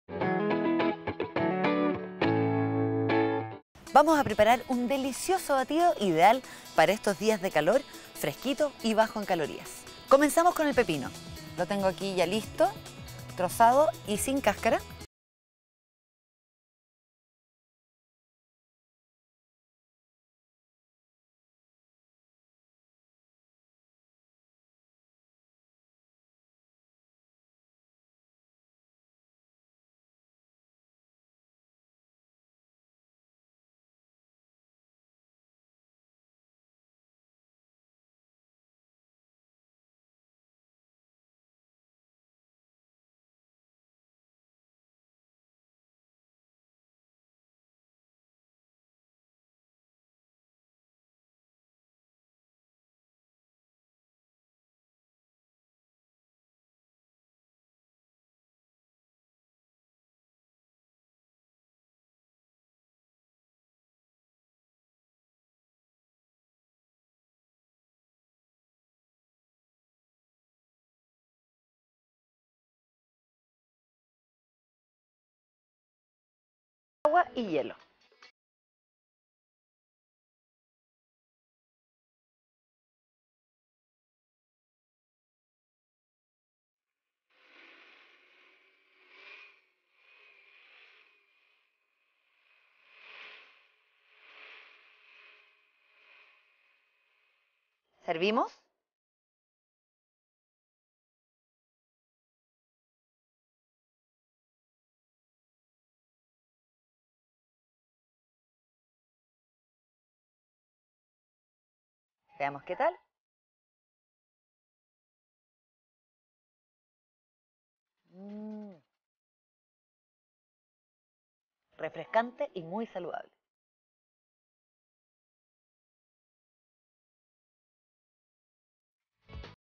BATIDO DE APIO, PEPINO Y MIEL / MANOS A LA OBRA
DISFRUTA DE ESTE RICO BATIDO!
INGREDIENTES:

100g de apio
100g de chía
2 naranjas
8 frutillas
1 pepino
1 trocito de jengibre
2  cucharadas de miel
Menta fresca